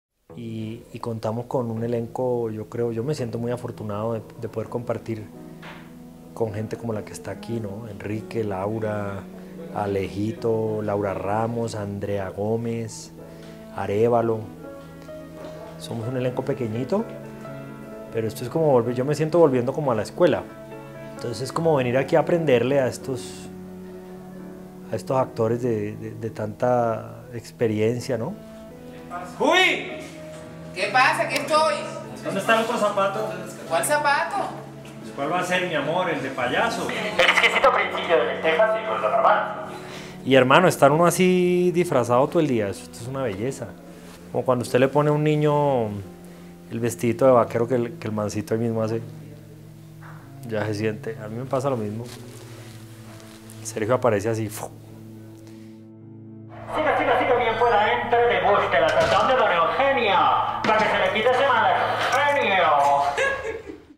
"Yo me siento aquí volviendo a la escuela " Andrés Parra
Lo dice de entrada, sin dudarlo, como si tuviera que decirlo antes de explotar: “Fue la película más difícil en la que he estado porque no había manera de salvarse. Fue un proceso terrible”. Así de categórico es Andrés Parra acerca del rodaje de Siempreviva. Pero no lo dice por disgusto, sino con una mezcla de admiración y de espanto, la misma que experimenta un alpinista recorrido cuando tiene de frente una montaña que parece superarlo y sin embargo logra coronar su cima.

“Yo creo que a todos nos pasó lo mismo: en el tema de los planos secuencia sufrimos mucho porque era muy difícil que todas las partes se sintieran satisfechas con el resultado. A unas no les gustaba su ángulo, otra pensaba que podía haberlo hecho mejor. Aunque fue un rodaje muy agradable en términos de grupo y del trato que hubo, en la parte actoral fue muy difícil. Si uno entraba en la cola de una escena al final tenía que tener una profunda sincronización y nada podía fallar. Todo era complicado”.

Esa estructura puso a sufrir a todos, reconoce Parra. “Había escenas en las que todos sentíamos que por fin lo habíamos hecho bien y aparecía un cable que no debía verse. En serio: como experimento actoral fue muy difícil. No entiendo cómo coño lo logramos. Pero lo hicimos gracias a las conversaciones entre actores, a las pausas y a los aportes del director. Yo, por mi lado, aprendí mucho de Laura García, de Enrique Carriazo, de la cubana Laura Ramos, de Fernando Arévalo, de todos. No solo aprendí, sino que al final lo disfruté”.

Pero ¿qué fue lo más difícil para Parra? “La construcción de mi personaje como Sergio. En el camino fui armándolo, con mucha ayuda de Enrique Carriazo y del director. Enrique fue quien vio la luz en cuanto a la relación de los dos personajes antagónicos (Sergio y Carlos) y fue gracias a ello que Sergio empezó a crecer, en parte por la enemistad y la competencia entre los dos. Enrique propuso cosas que hicieron que el personaje se reforzara y hubo una química que enriqueció a uno y otro. Hoy puedo decir que es un personaje al que quiero mucho y que responde a lo que he venido pidiendo: a alguien que se distancia de los malos y los buenos, y que es trágico porque refleja al ser humano en decadencia”.